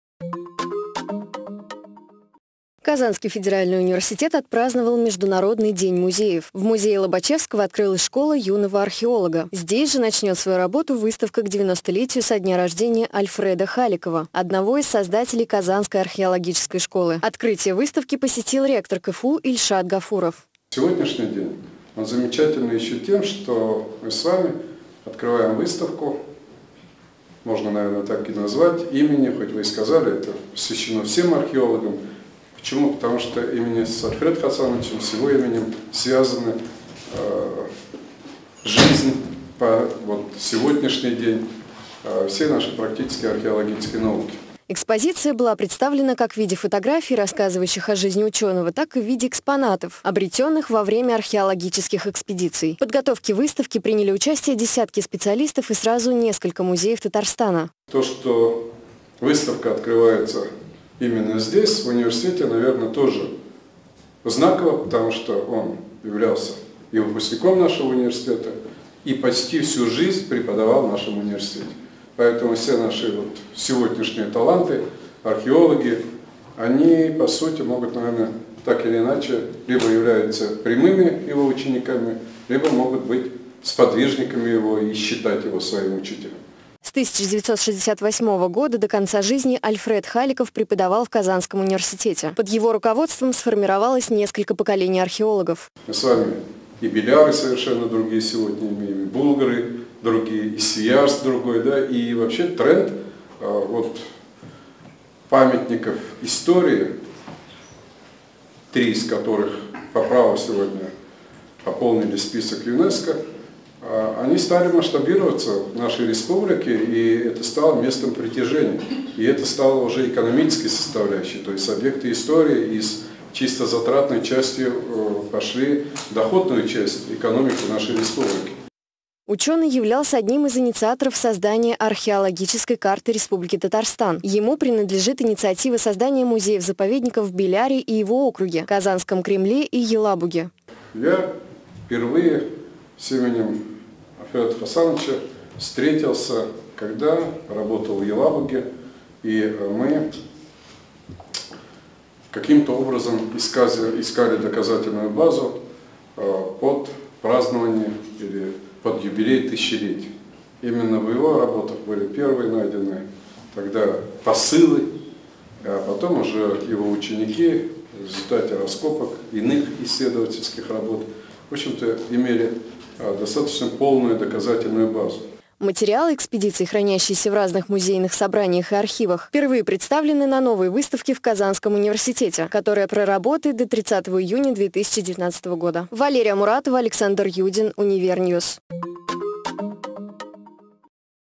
Выставка к 90-летию со дня рождения Альфреда Халикова
18 мая, в Международный день музеев Казанский федеральный университет провёл череду памятных мероприятий. Одним из них стало открытие выставки, посвященной 90-летию со дня рождения Альфреда Халикова. Ее посетили ученики и дети прославленного археолога Татарстана.